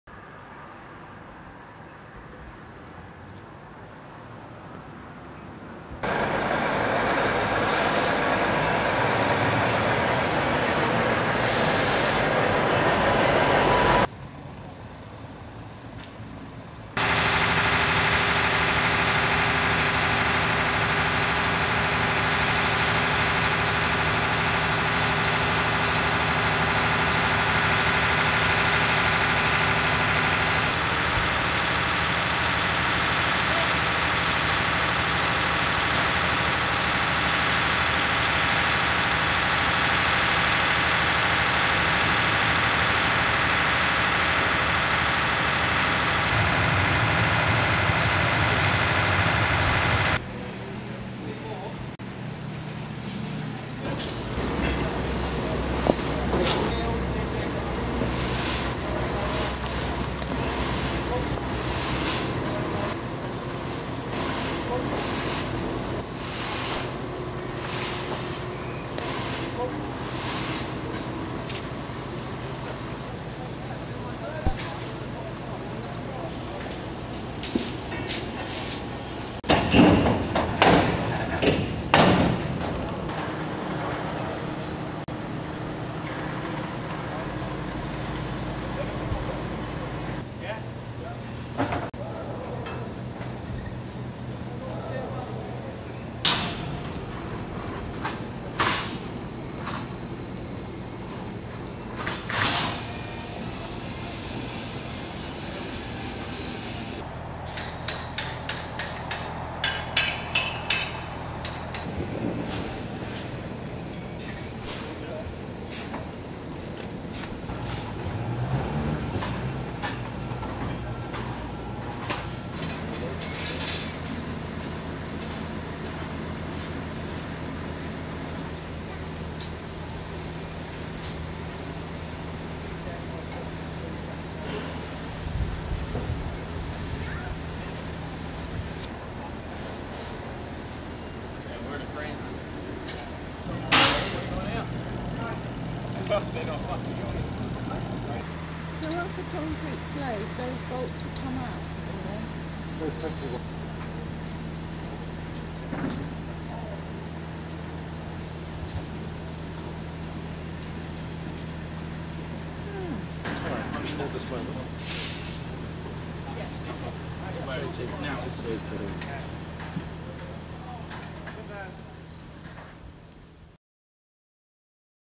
STAGE 20 LATTICE BEAMS - THE ENDGAME
Video shows the iconic lattice beams being replaced on the Whitchurch Bridge after they were refurbished. The operation started on 7th July and was completed on the early evening of 9th July 2014.